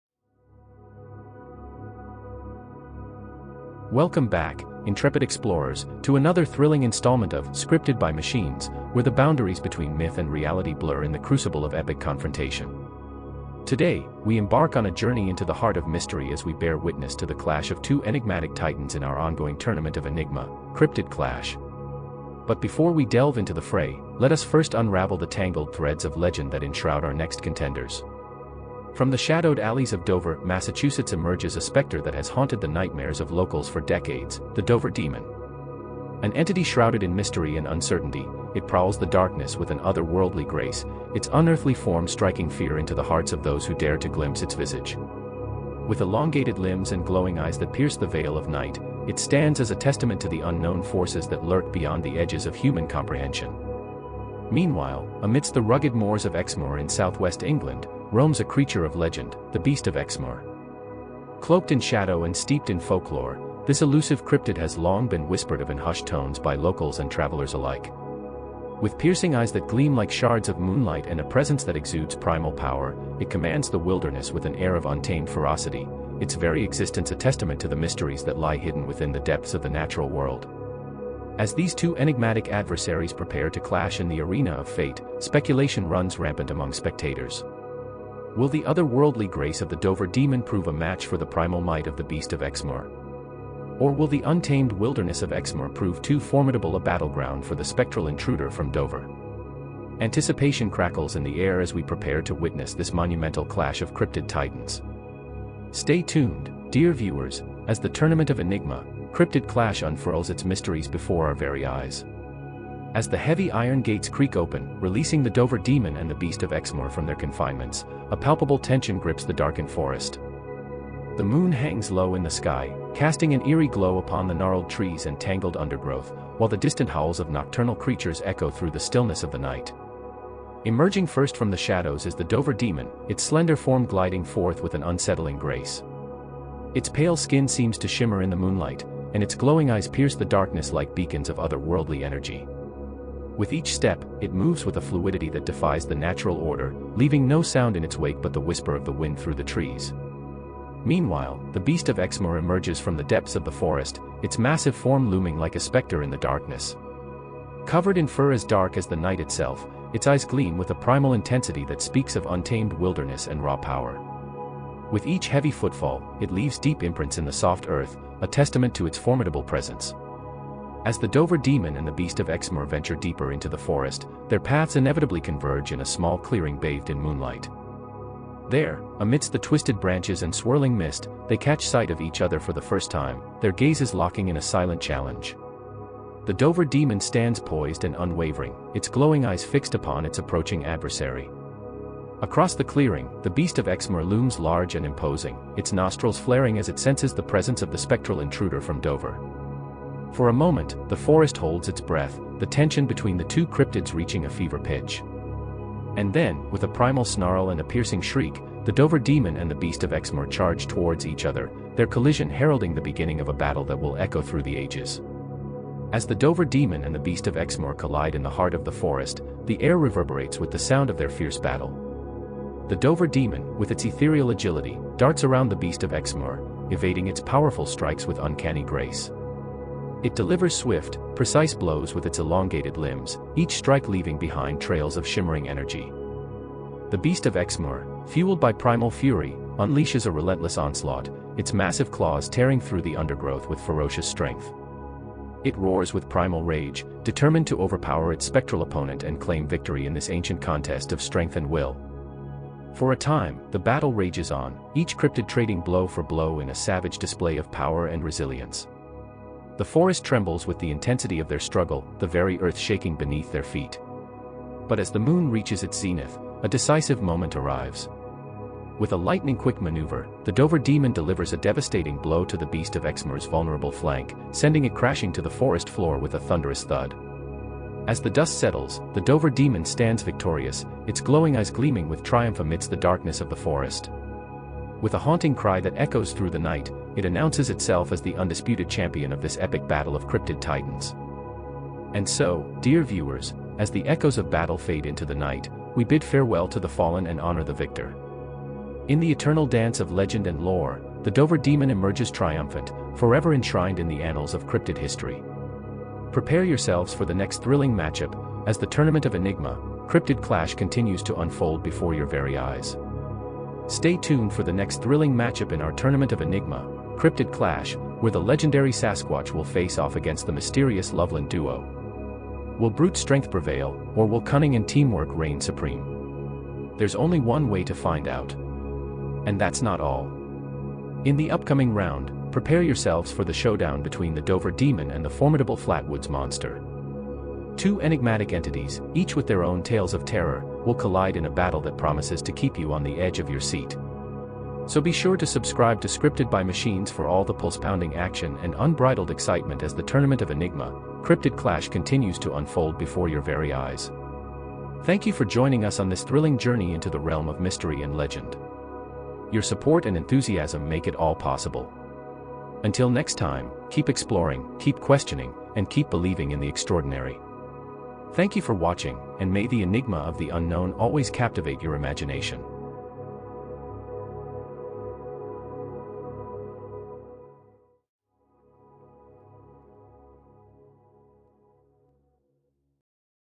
Dover Demon Vs Beast of Exmoor
Prepare yourself for the ultimate showdown in the Cryptid Tournament! Sixteen legendary creatures from folklore and myth have gathered to compete in a series of epic battles, each vying for the title of ultimate cryptid champion.

From the elusive Mothman to the bone-chilling Wendigo, and from the mysterious Chupacabra to the fearsome Jersey Devil, witness as these iconic beings clash in a tournament like no other.

Who will emerge victorious and claim the coveted title of the most powerful cryptid? Follow along as the battles unfold, alliances are forged, and rivalries ignite in an electrifying spectacle scripted by machines.